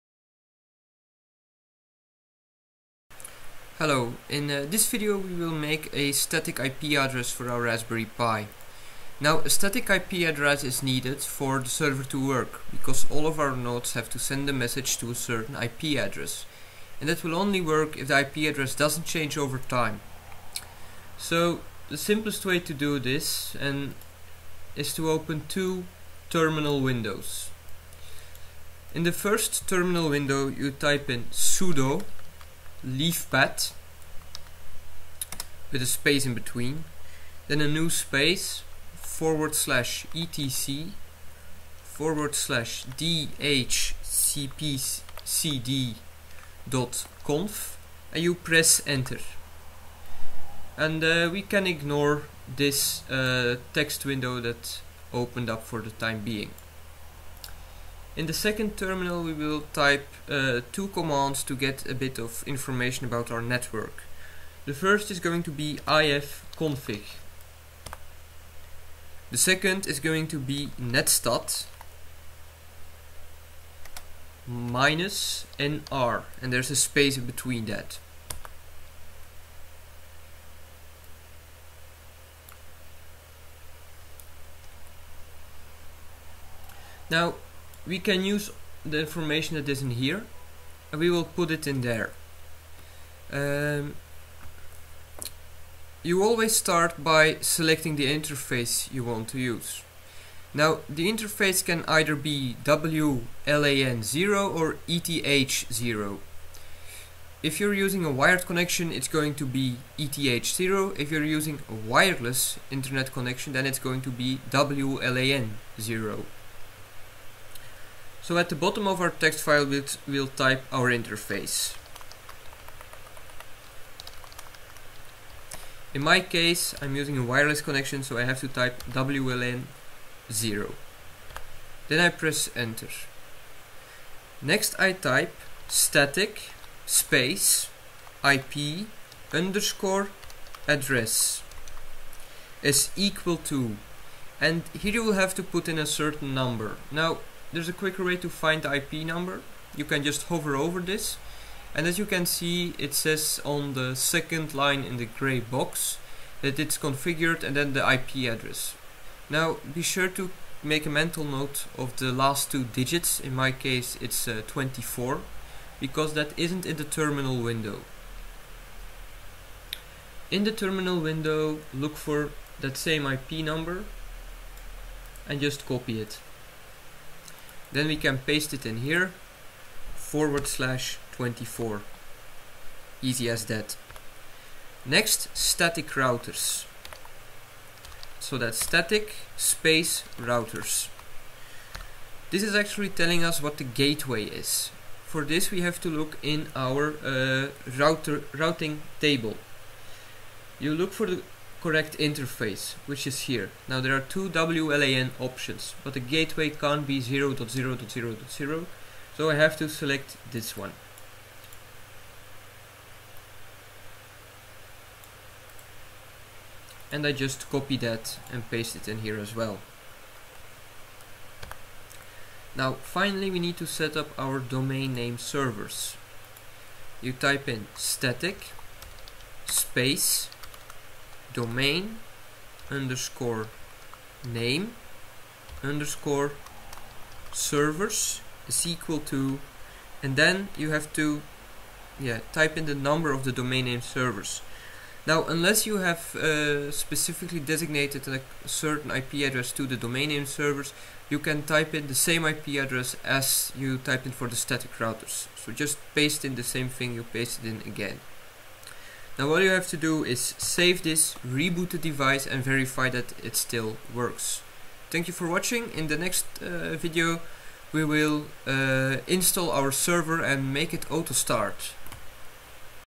Pi Lib Server: Setting a static IP address.
In this video we will set a static IP address for our server.